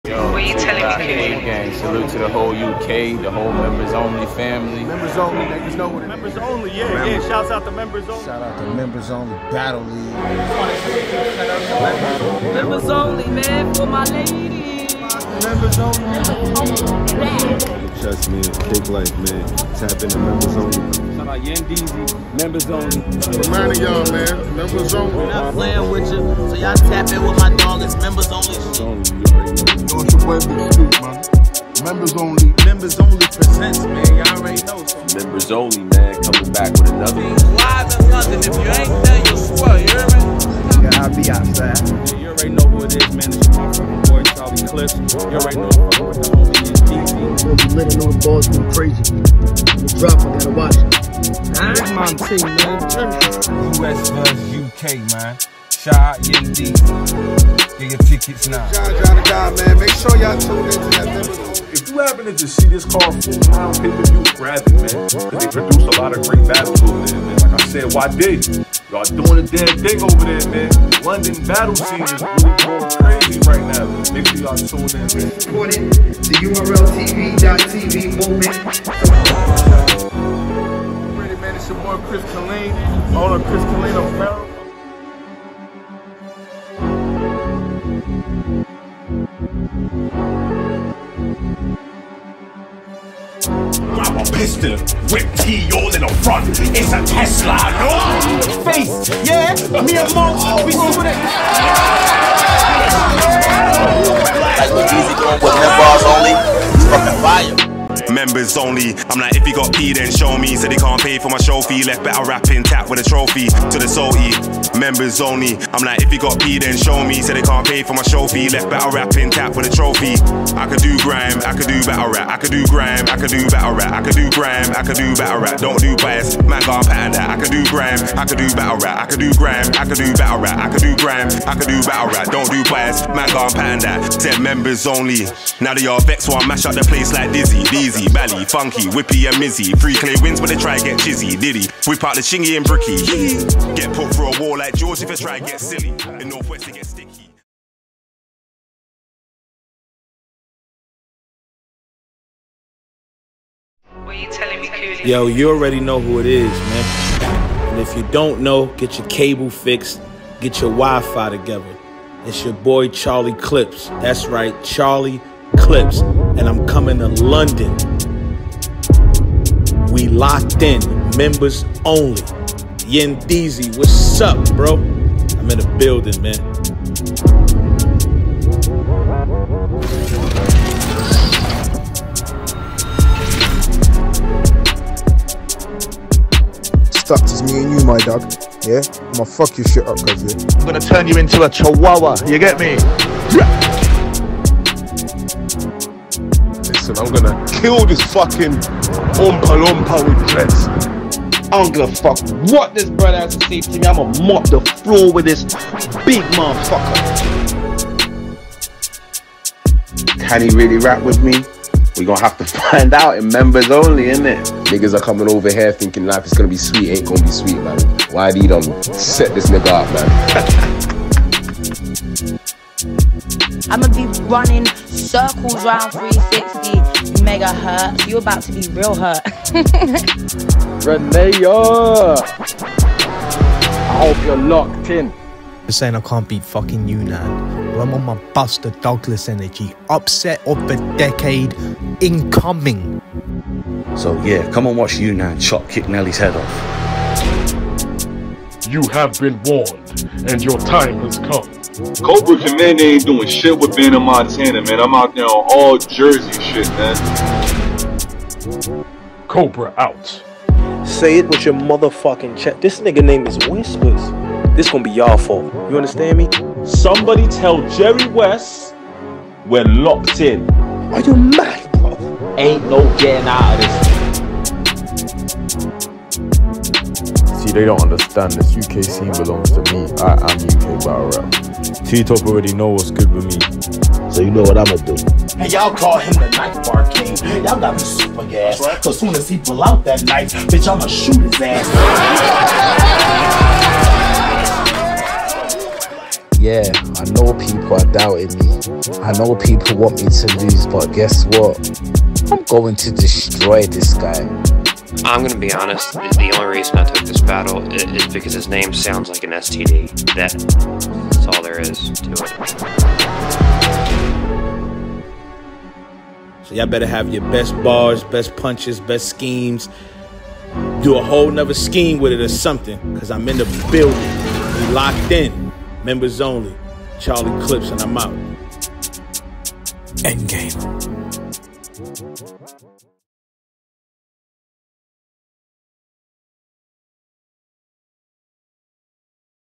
Final Trailer | Locked In🔒Hosted By Charlie Clips🇺🇸 | Membarz Only
Save the date for the most epic battle rap event of the year!

 "LOCKED IN" Hosted by Wild 'N' Out star & Battle Rap Legend Charlie Clips and our very own Grime Legend ChronikSlewDem.

Grime Legend Funky Dee goes head-to-head with URLTV & Bags & Bodies viral sensation Anderson Burrus.

UK BOTY, Don't Flop Champion and Beta Squad Star Gemin1 takes on URLTV’s star & UFF Champion T-Top.

The NLC Process Champion Whispers is up against URLTV’s Summer Madness 13 star Jerry Wess.

Jump Off TV Legend Cobra Commander prepares for URLTV’s Crucible star Bandit Montana.

Beast Anglia’s 5x BOTY Kick Nelly stands the grand test of Don’t Flop & Premier Battles Champion Unanymous. + many more

Date: Saturday, April 27, 2024  Time: 12:00 PM - 6:00 PM  Venue: Brixton Jamm, 261 Brixton Road, Brixton, London SW9 6LH, United Kingdom

Secure your spot at this high-energy event now! Don't miss out on the electrifying showdown.

Get your tickets at Resident Advisor, purchase the Ppv and join the Membarz Only Battle Rap League community.

Facebook: Membarz Only Battle Rap League

Be prepared to witness history in the making! See you at "LOCKED IN" - the ultimate battle rap experience.